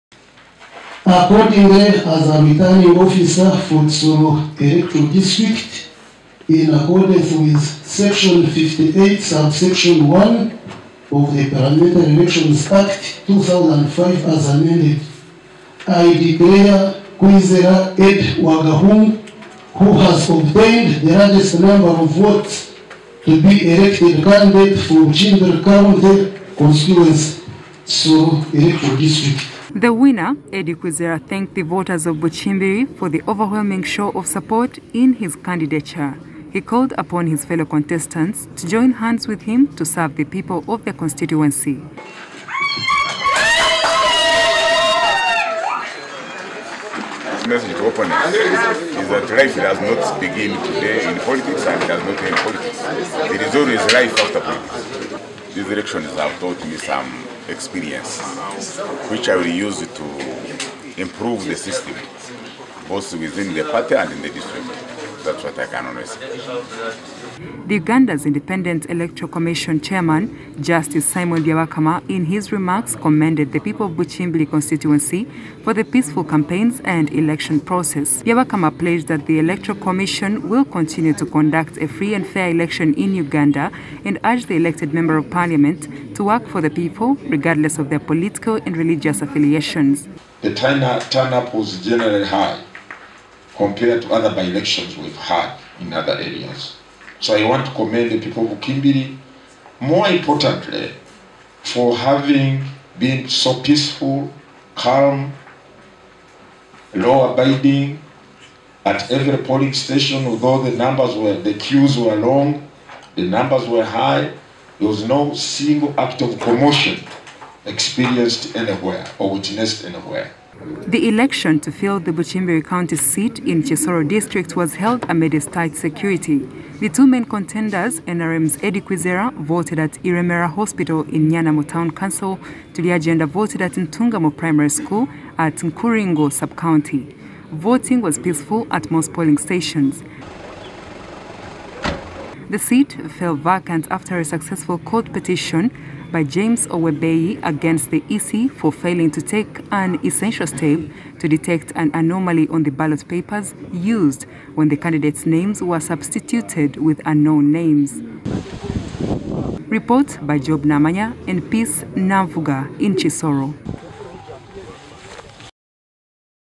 NRM’s EDDIE KWIZERA WINS BUKIMBIRI ELECTIONS
NRM flagbearer Eddie Kwizera Wagahungu has been declared the winner of the Bukimbiri constituency by-election with 12,214 votes.
Independent candidate Asgario Turyagyenda came second polling 10,105 votes. Edward Mutabaazi, an independent candidate, got 308 votes and Forum for Democratic Change, James Owebeyi, got 208.
The results were declared by Kisoro district returning officer Dan Nayebare at the Hope Mission tally centre in Rubuguri town council at 10:00am today.